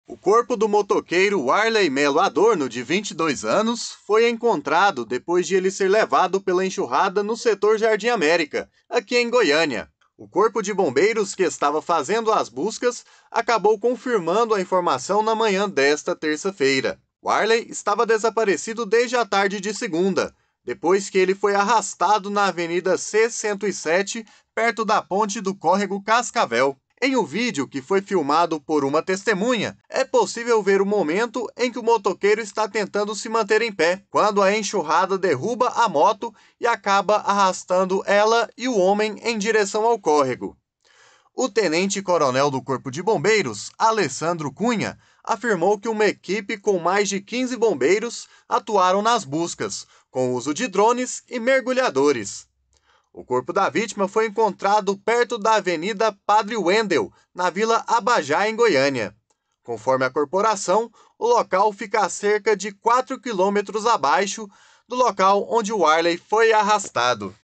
Corpo de motociclista arrastado por enxurrada durante temporal é encontrado em Goiânia
Apresentação: Carlos Eduardo Prado

Warley Melo Adorno, de 22 anos, estava desaparecido desde a tarde de segunda-feira. Bombeiros precisaram usar drones e mergulhadores durante buscas.